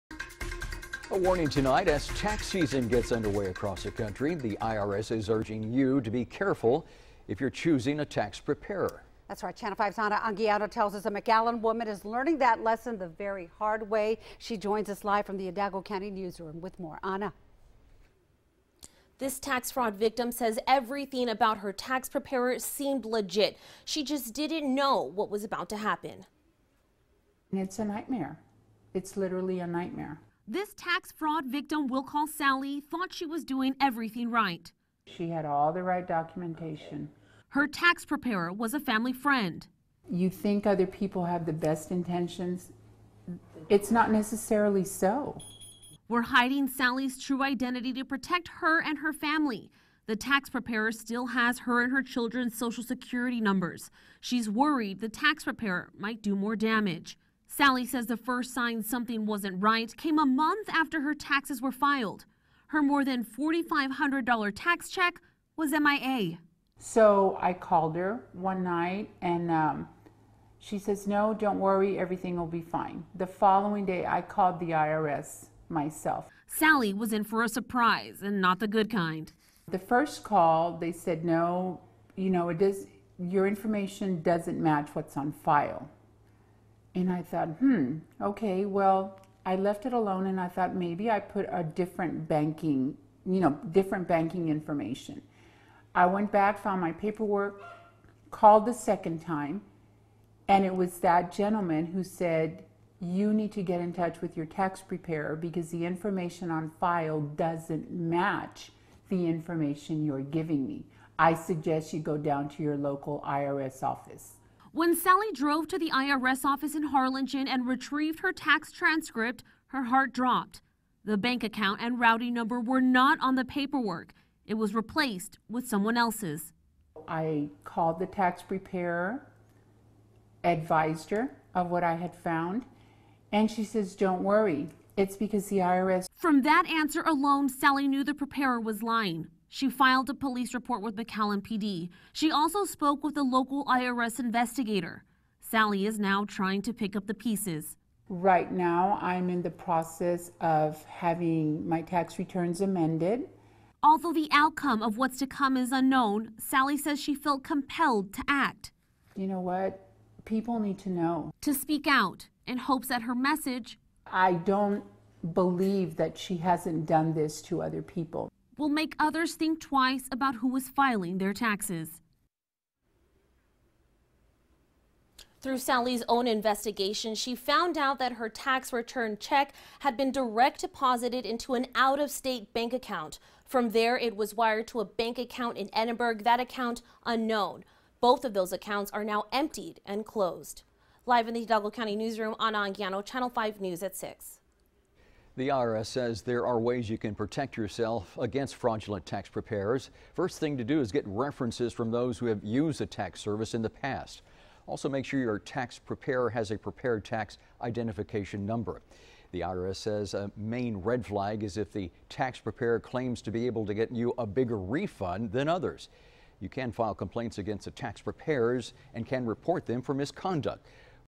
RGV Victim Speaks Out about Tax Fraud
The woman said she worries the tax preparer might do more damage.